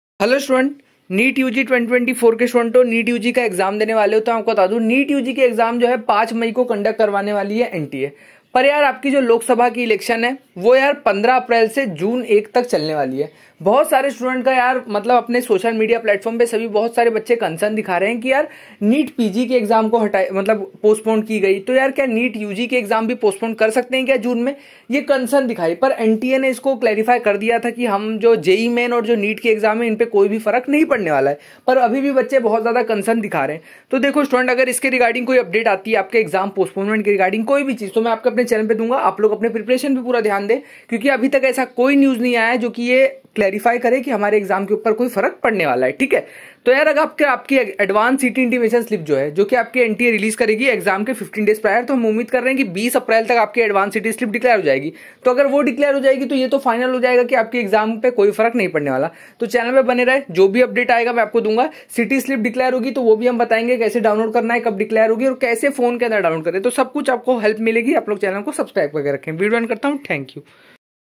" ALERT 🚨 NEET-UG 2024 :- EXAM POSTPONEMENT UPDATE 🤔 ADVANCE CITY SLIP/ADMIT CARD DATE."
"ALERT 🚨 NEET-UG 2024 EXAM :- CORRECTION WINDOW DATE 📅 MISTAKE IN APPLICATION FORM UPDATE."